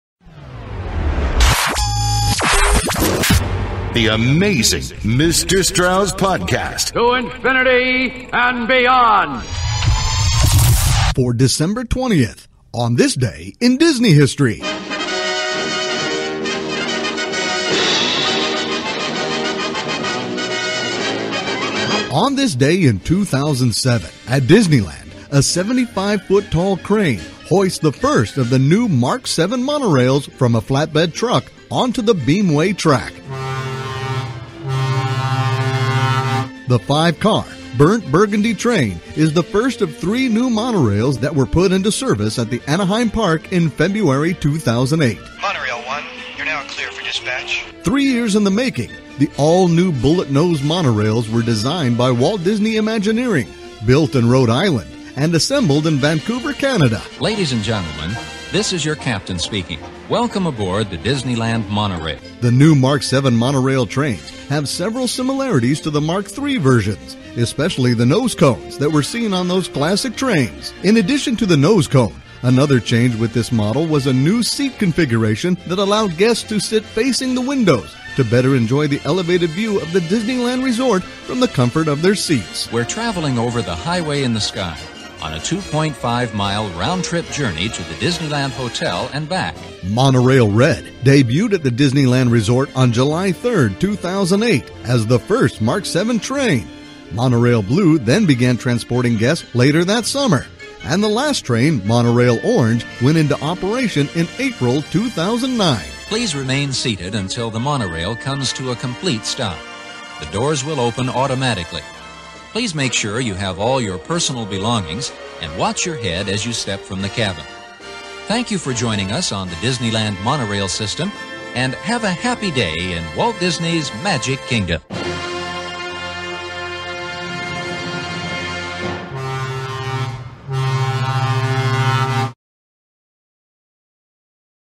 On This Day in Disney History - Dec 20th
For December 20th...On This Day in Disney History in 2007 at Disneyland, a 75 foot tall crane hoists the first of the new Mark VII Monorails from a flatbed truck onto the beamway track.  The five-car, burnt burgundy train, is the first of three new monorails that were put into service at the Anaheim park in February 2008.  Three years in the making, the all-new bullet-nosed monorails were designed by Walt Disney Imagineering…built in Rhode Island…and assembled in Vancouver, Canada.  The new Mark VII Monorail trains have several similarities to the Mark III versions…especially the nose cones that were seen on those classic trains.  In addition to the nose cone, another change with this model was a new seat configuration that allowed guests to sit facing the windows to better enjoy the elevated view of the Disneyland Resort from the comfort of their seats.  Monorail Red debuted at the Disneyland Resort on July 3rd, 2008 as the first Mark VII train.  Monorail Blue then began transporting guests later that summer.  And the last train, Monorail Orange, went into operation in April 2009.